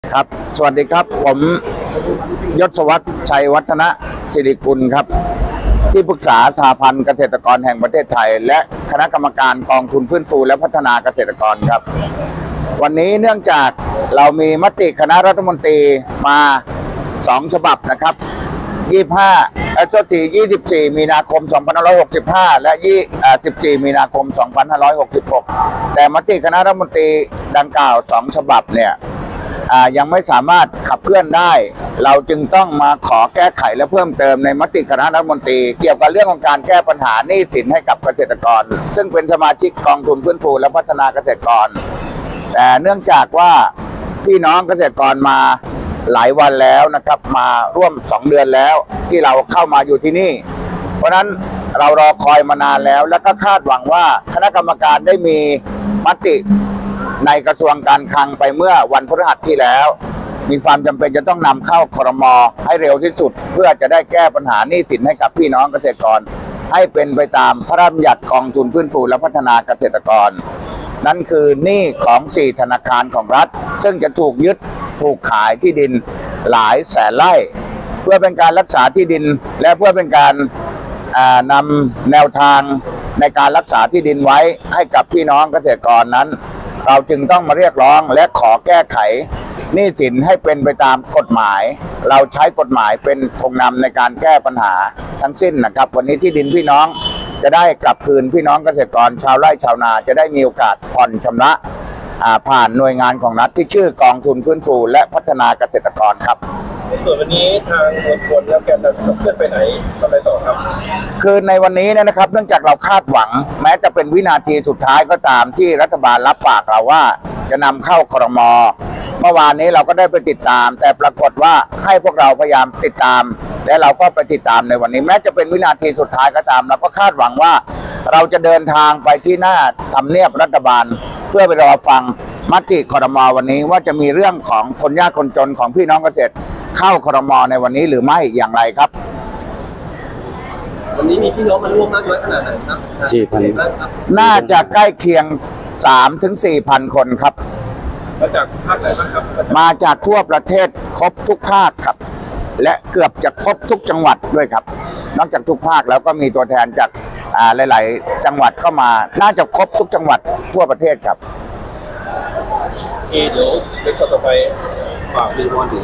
สหพันธ์เกษตรกรฯ นำเกษตรกร 4,000 ราย บุกทำเนียบรัฐบาล จี้รัฐแก้ปัญหาหนี้สินเพิ่มเติม หลังรอคำตอบนาน
สหพันธ์เกษตรกรฯ นำเกษตรกร 4,000 ราย บุกทำเนียบรัฐบาล จี้รัฐแก้ปัญหาหนี้สินเพิ่มเติม หลังรอคำตอบนานหลายวัน
วันนี้ 5 พฤศจิกายน 2567 สหพันธ์เกษตรกรแห่งประเทศไทย สกท จากทุกภูมิภาค นำโดยนายยศวัฒน์ ชัยวัฒนสิริกุล ที่ปรึกษาใหญ่ของสหพันธ์ฯ ได้พากลุ่มเกษตรกรกว่า 4,000 คน เดินทางไปยังทำเนียบรัฐบาล เพื่อเรียกร้องให้รัฐบาลเร่งดำเนินการแก้ไขปัญหาหนี้สินและปรับโครงสร้างหนี้เพิ่มเติม หลังรอคำตอบจากการชุมนุมที่กระทรวงการคลังมาหลายวัน
     นายยศวัฒน์ ชัยวัฒนสิริกุล ที่ปรึกษาสหพันธ์เกษตรกรแห่งประเทศไทยและคณะกรรมการกองทุนฟื้นฟูและพัฒนาเกษตรกร กล่าวว่า จากมติคณะรัฐมนตรีเมื่อ วันที่ 24 มีนาคม 2565 และ 14 มีนาคม 2566 ยังไม่สามารถขับเคลื่อน เพื่อแก้ปัญหาหนี้สินของพี่น้องเกษตรกรที่เป็นสมาชิกกองทุนฟื้นฟูฯ โดยเน้นการรักษาที่ดินไม่ให้ถูกยึดขาย ซึ่งเป็นหนี้สินที่พี่น้องเกษตรกรได้กู้จาก 4 ธนาคารรัฐ จึงทำการเดินขบวนขับเคลื่อนเรียกร้อง ให้มีการแก้ไขปัญหานี้เพื่อให้เกษตรกรสามารถผ่อนชำระหนี้ผ่านกองทุนฟื้นฟูฯ และรักษาที่ดินไว้ได้
     ยศวัฒน์ ชี้แจงว่า "พี่น้องเกษตรกรรอคอยมานานกว่า 2 เดือน โดยตั้งแต่เข้ามาอยู่ที่หน้ากระทรวงการคลัง พวกเราต้องการให้รัฐบาลรับปากว่าจะนำปัญหานี้เข้าสู่การพิจารณาของคณะรัฐมนตรีอย่างเร่งด่วน"
     เรียกร้องให้รัฐบาลช่วยเหลือพี่น้องเกษตรกรให้ได้รับโอกาสผ่อนชำระหนี้ ยศวัฒน์และกลุ่มเกษตรกรยังเรียกร้องให้รัฐบาล โดยเฉพาะนายกรัฐมนตรีและรัฐมนตรีกระทรวงการคลัง ได้รับรู้ถึงความเดือดร้อนและยื่นขอให้การช่วยเหลือพี่น้องเกษตรกรเป็นไปตามกฎหมาย โดยเฉพาะอย่างยิ่งหนี้สินในครัวเรือนที่กระทบต่อคุณภาพชีวิตของคนในชุมชนเกษตร
     ในช่วงเช้าวันนี้ กลุ่มเกษตรกรได้เดินทางไปยังทำเนียบรัฐบาลเพื่อรอฟังการพิจารณามติจากคณะรัฐมนตรี โดยมีเกษตรกรจากทั่วประเทศเข้าร่วมเป็นจำนวนมาก รวมถึงจากภาคอีสานและภาคอื่น ๆ ซึ่งตั้งใจจะต่อสู้เพื่อสิทธิ์ในการรักษาที่ดินของตน
     หวังเป็นวินาทีสุดท้ายที่รัฐบาลจะตอบสนองตามนโยบายช่วยเหลือเกษตรกร นอกจากการแก้ปัญหาหนี้สิน ยศวัฒน์ยังเน้นย้ำถึงความสำคัญของการรับฟังจากรัฐบาลเพื่อช่วยพี่น้องเกษตรกรให้สามารถมีชีวิตที่มั่นคงและมีโอกาสในการผ่อนชำระหนี้ โดยไม่เพียงแค่การขอความช่วยเหลือจากรัฐบาลแต่เพียงอย่างเดียว แต่พี่น้องเกษตรกรยืนยันว่าจะผ่อนชำระคืนตามกำหนด